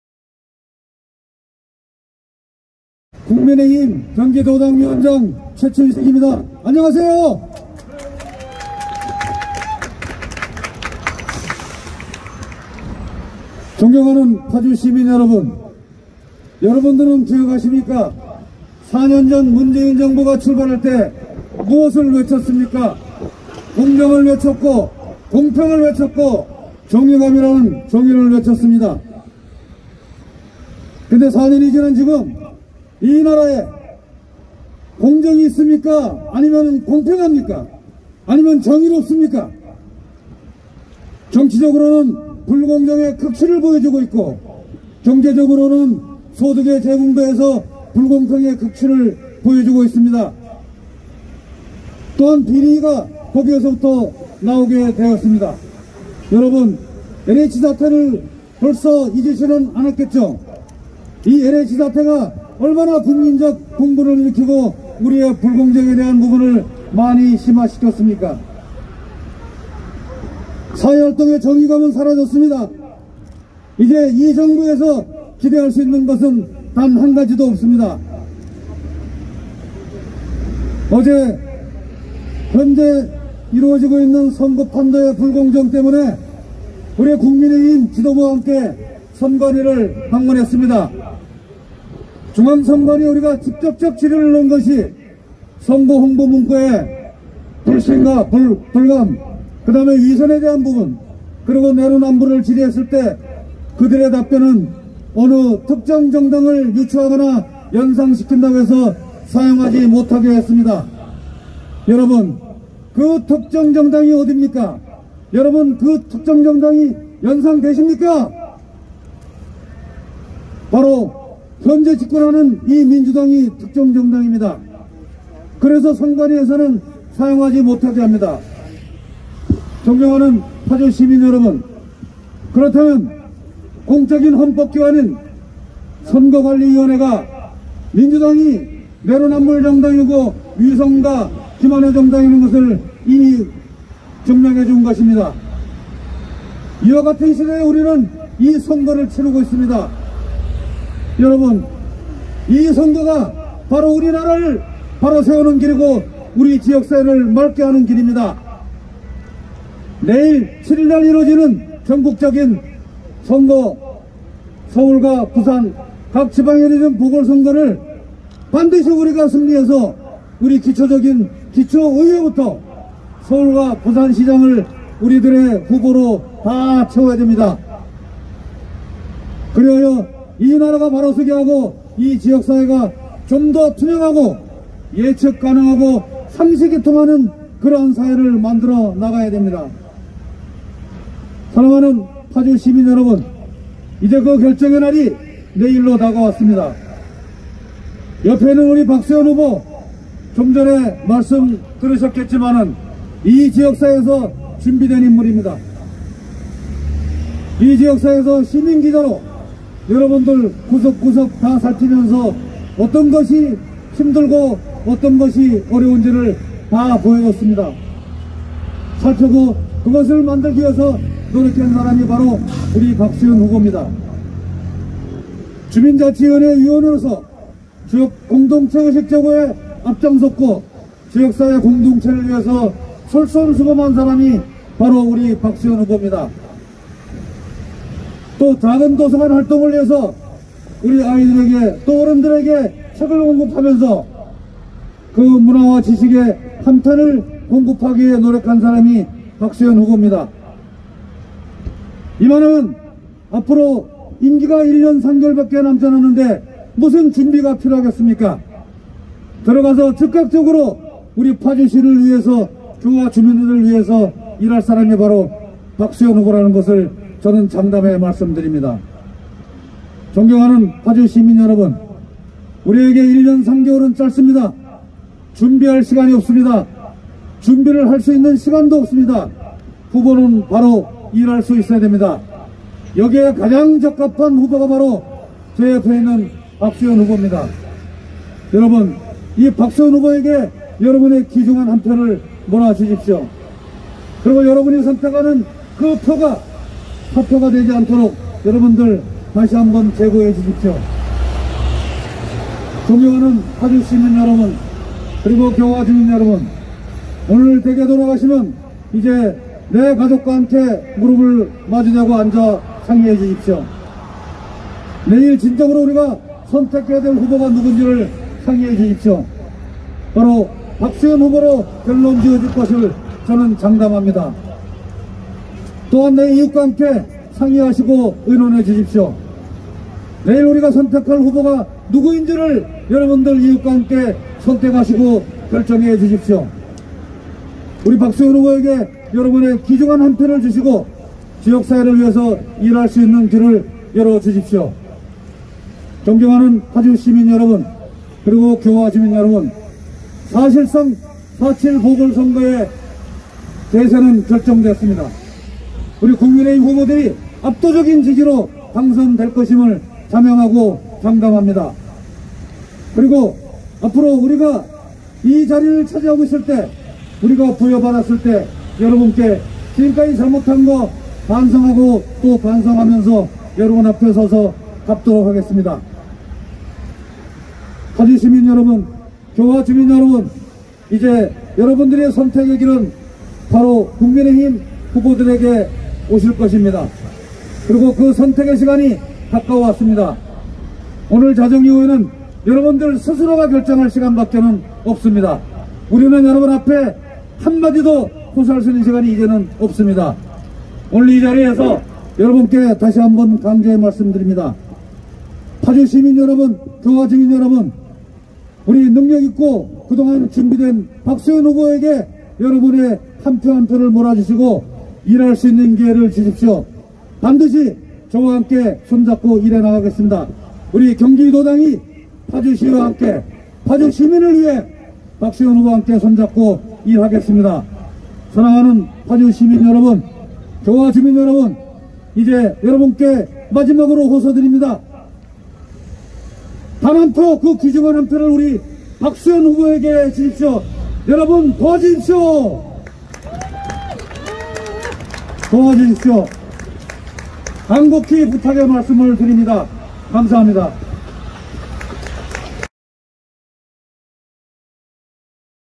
최춘식 경기도당 위원장/포천,가평 국회의원/ 파주 박수연 시의원 후보 지원유세/직캠
최춘식 포천,가평 국회의원/경기도당 위원장
파주시 박수연 시의원 후보 지원유세